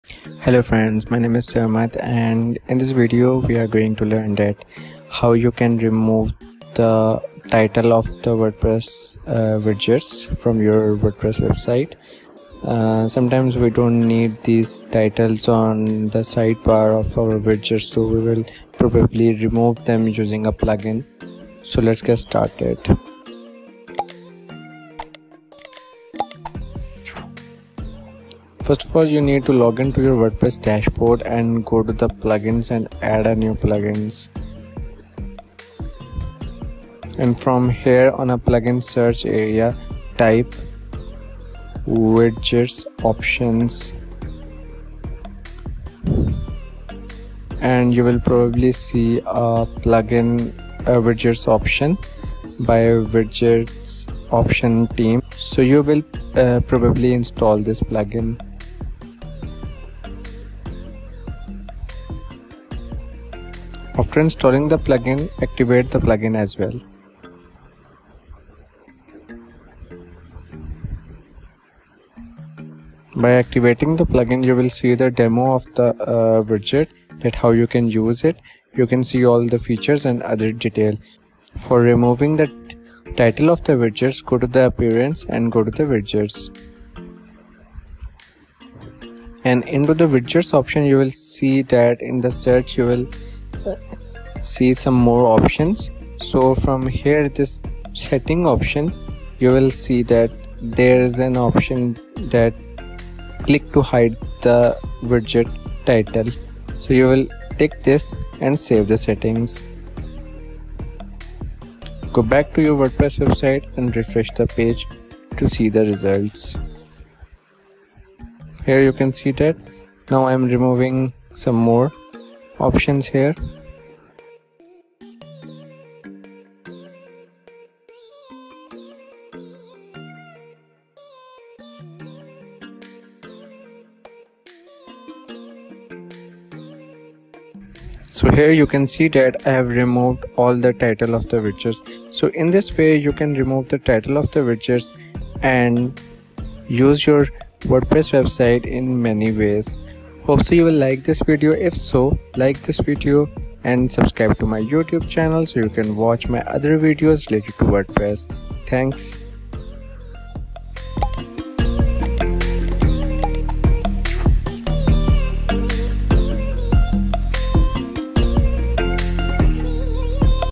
How to Hide the Title of WordPress Widgets
Do you want to hide the title of a widget on your WordPress site? WordPress has a tool to make it easy to drag and drop dynamic content into your theme to create unique layouts. However, if you need to hide the title it previously required knowledge of HTML and CSS. In this guide, we'll show you how to easily hide the widget title in WordPress without any code.

For this guide we will be using the Widget Options plugin you can find below:

With the plugin installed and activated you should now be able to go under Appearance, Widgets in your admin area. Under each widget, there should be additional tabs in each widget and under the cog icon, you will find the setting to hide widget titles.

Now, as long as you save the settings you should no longer see the widget titles on your site for the ones you've hidden.